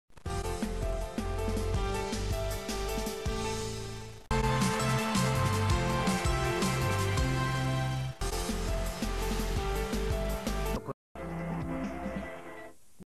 TV Nova - znělky 20 let - 2014
Znělky k 20 letem TV Nova z roku 2014
Bohužel jsem našel jen tyto čtyři, dvě nejsou kompletní